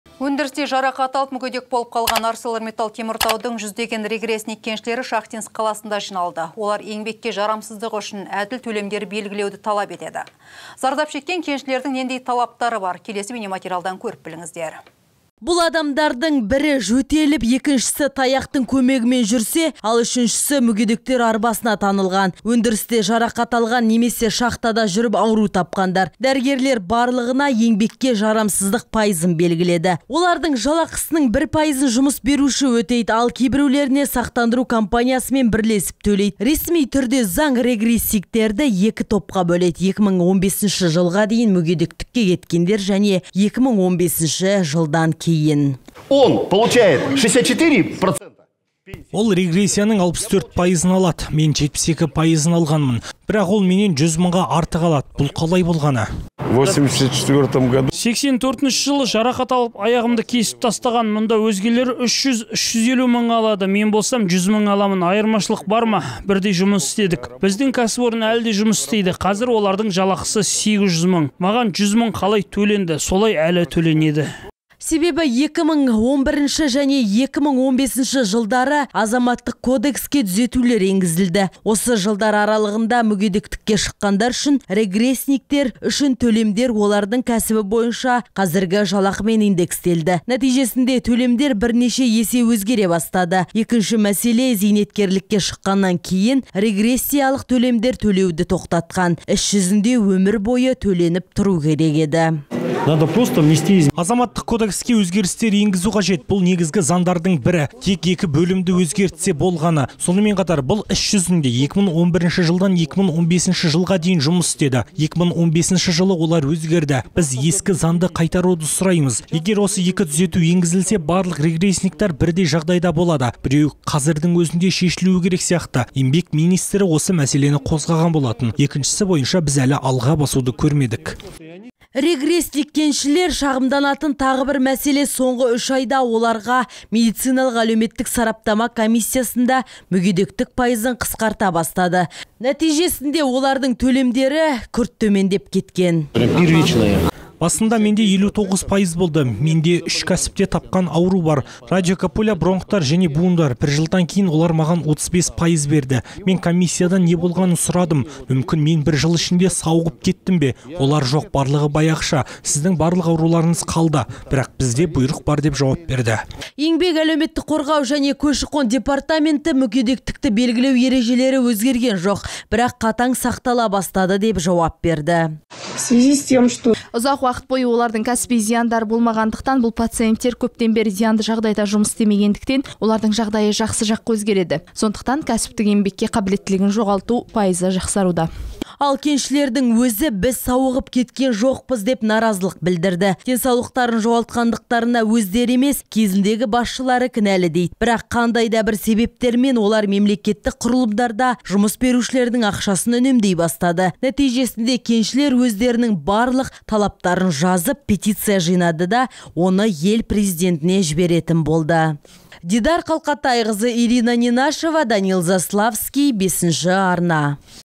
Олар еңбекке жарамсыздығы үшін әділ төлемдер белгілеуді талап етеді.
Өндірісте жарақат алып, мүгедек болып қалған АрселорМитал Теміртаудың жүздеген регрессник-кеншілері Шахтинск қаласында жиналды.  Зардап шеккен кеншілердің нендей талаптары бар, келесі бейнесюжеттен көріңіздер.